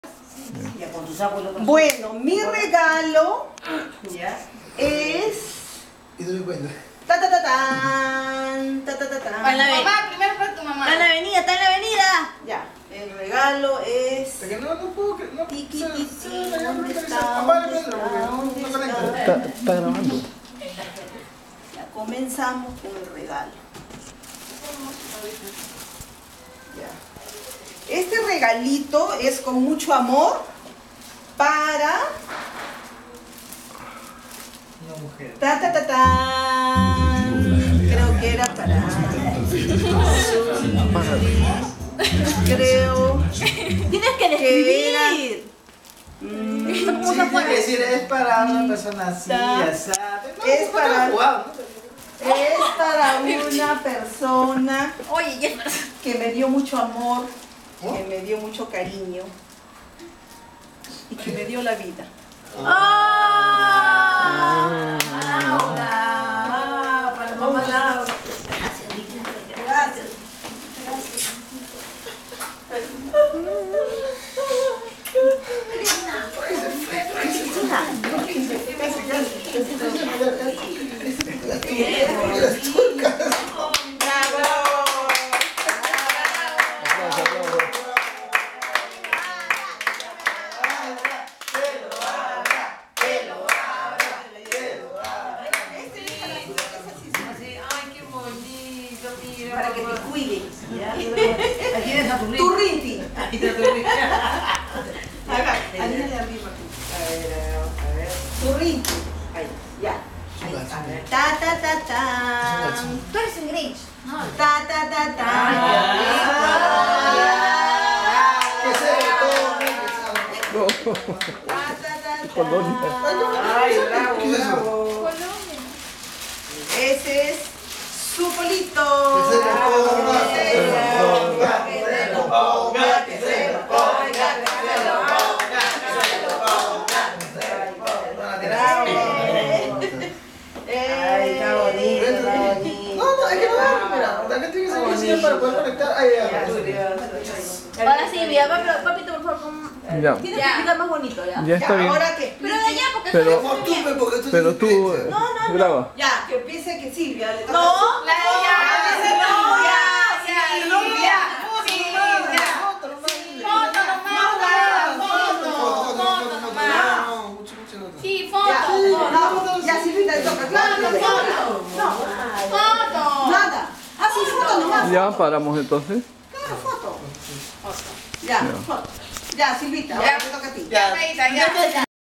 ENTREGA DE REGALOS.
ENTREGA DE REGALOS EN CASA, REALIZADO EL DÍA 25 DE DICIEMBRE, A LAS 5.00 PM. AÑO 2016.